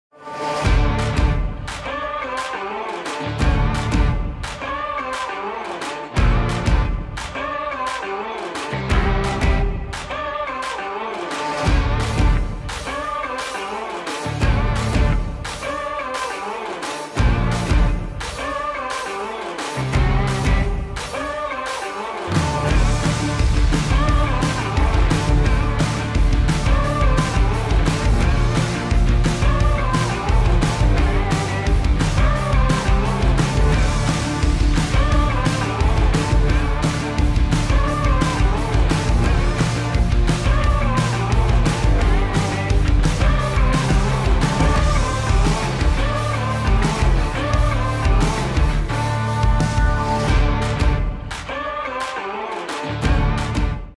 Part 1: The Magic Continues with Lagoon
Discover in video a first sketch of the latest from the Lagoon family.
See you soon to find out more!